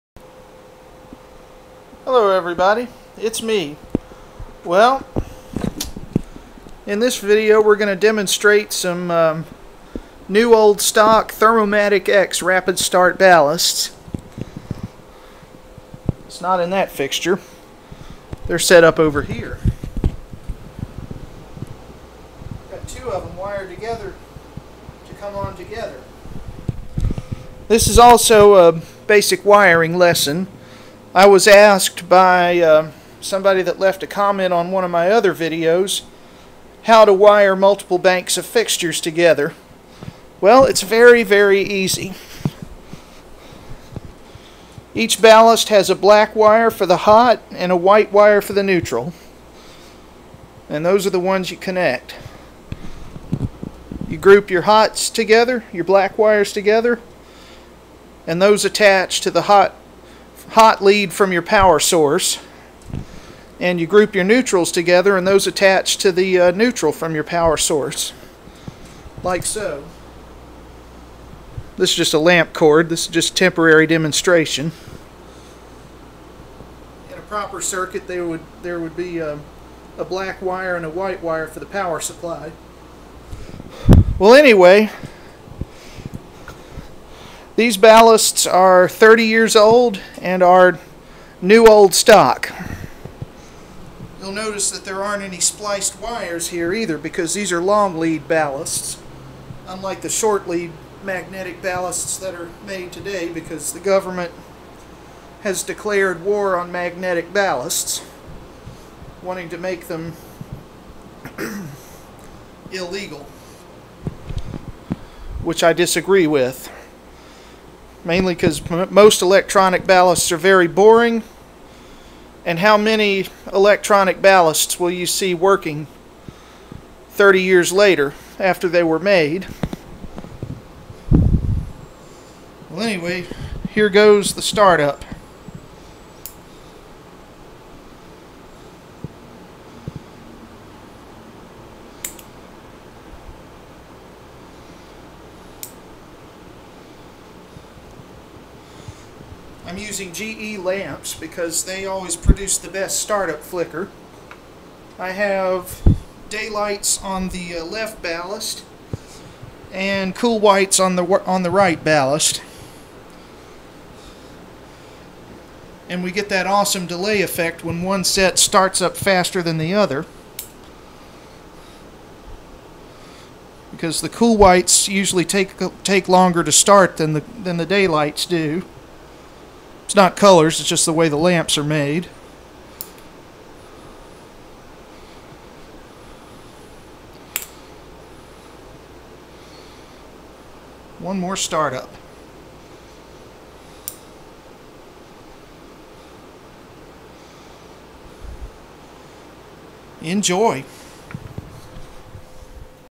NOS Thermomatic X Rapid Start Ballasts Starting Up
Here is my latest Ebay find. NOS Universal Thermomatic X rapid start F40 ballasts! Long leaded and the blue label ones. The lamps on the left are my GE Daylights. The ones on the right are my Ace Hardware (GE manufactured) F40CW Shoplight lamps. There are actually two more ballasts just like them not shown. I just ran out of lamp holders. Not to worry. I predict an eight lamp startup with all four ballasts very soon. Also there is a basic wiring tutorial for banking fluoro fixtures together. One thing I forgot, TURN OFF THE POWER BEFORE DOING ANY WIRING! 120 volts is VERY DANGEROUS!!! I want this to be a lesson in wiring, not a lesson on how to electrocute yourself. I am not responsible for any injury or property damage resulting from attempting this.